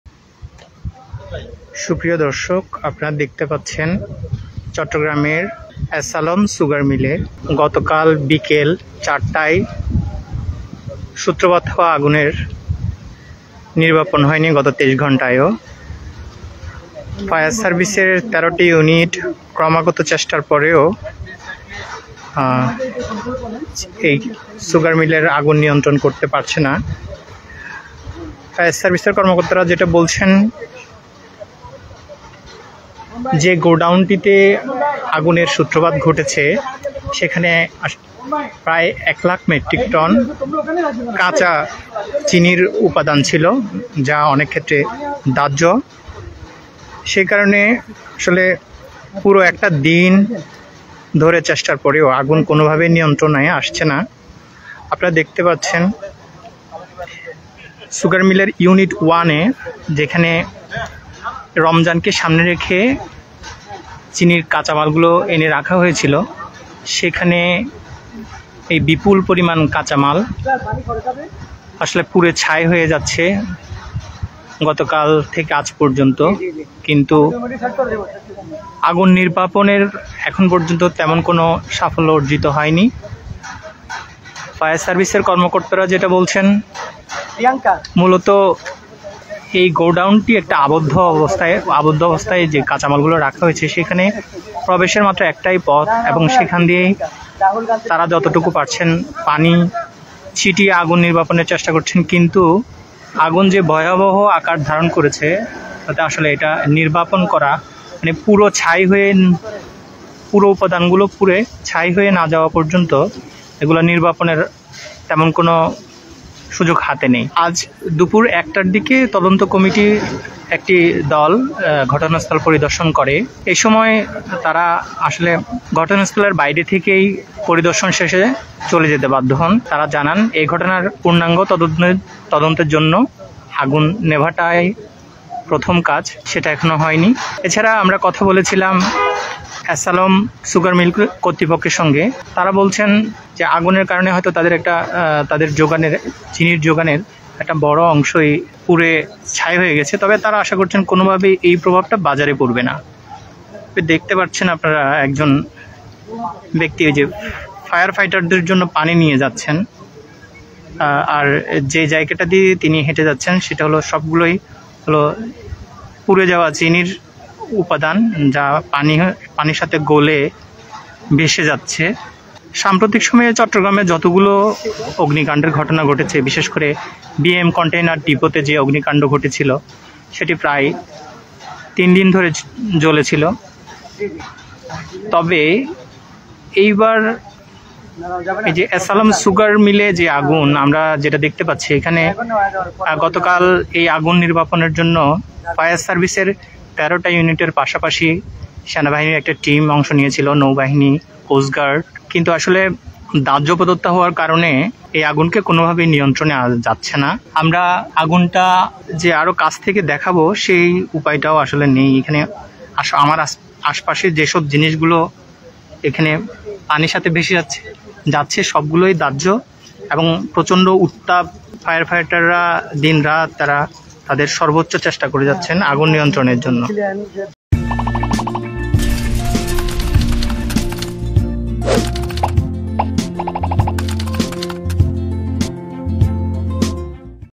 ২৩ ঘণ্টা ধরে জ্বলছে চট্টগ্রামের এস আলম সুগার মিলের আগুন | Jago News
২৩ ঘণ্টা ধরে জ্বলছে চট্টগ্রামের এস আলম সুগার মিলের আগুন